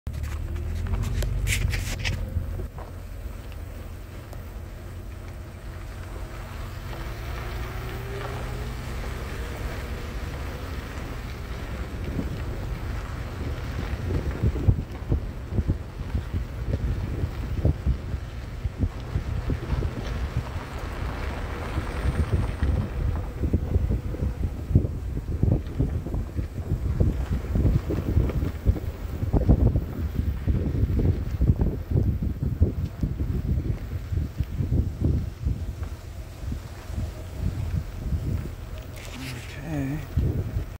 a ride thru the marsh
Riding in the marsh on a canal bank